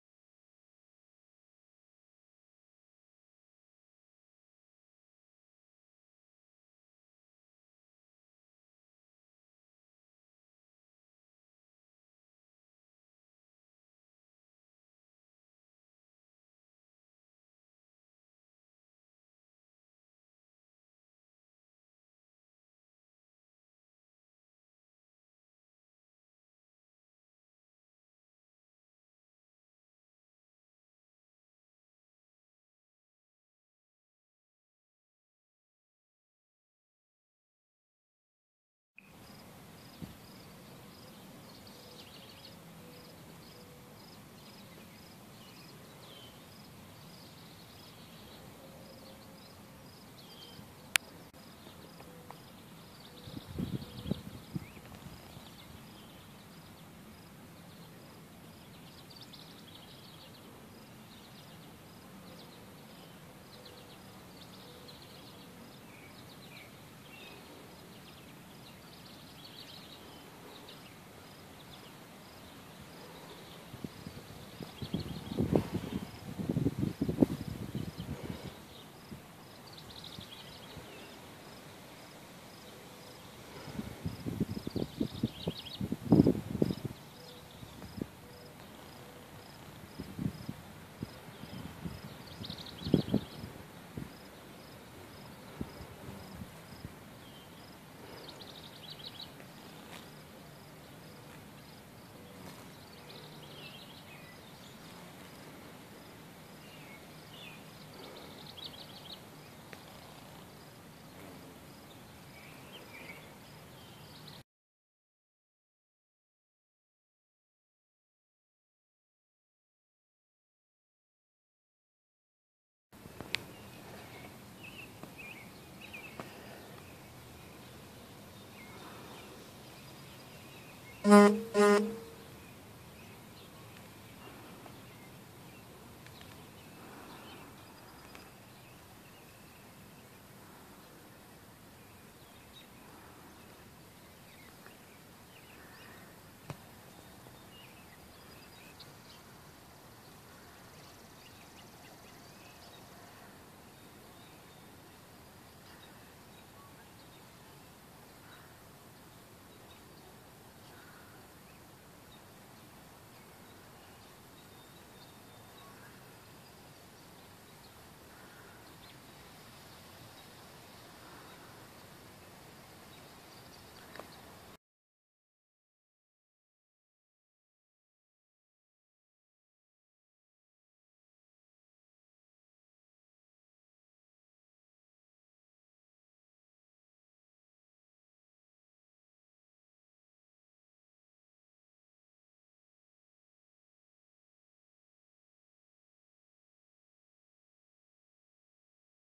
Wasps: The Original Organic Pesticide
Paper wasps forage upon and eat pests of broccoli and cabbage. Here, I show video of Polistes metricus capturing a caterpillar that was foraging on a broccoli plant. Then, I show how she feeds that prey to her larvae back at the nest.